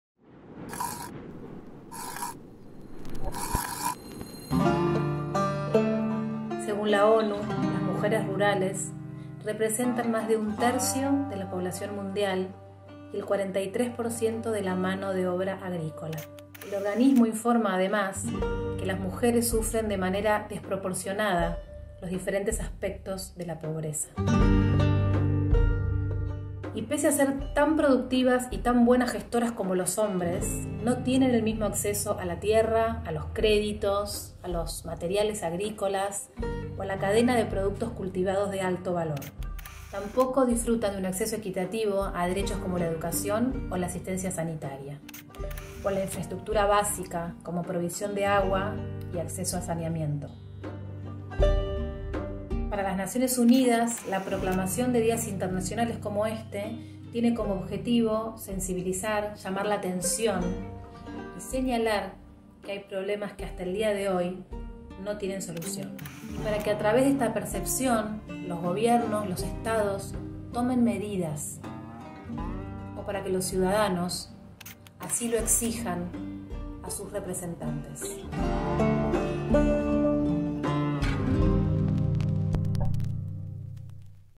HistoriaAlDía | 15 de octubre: Día Internacional de las Mujeres Rurales
El Día Internacional de las Mujeres Rurales se estableció para destacar el papel crucial de la mujer rural e indígena en la promoción del desarrollo agrícola y rural, la mejora de la seguridad alimentaria y la erradicación de la pobreza.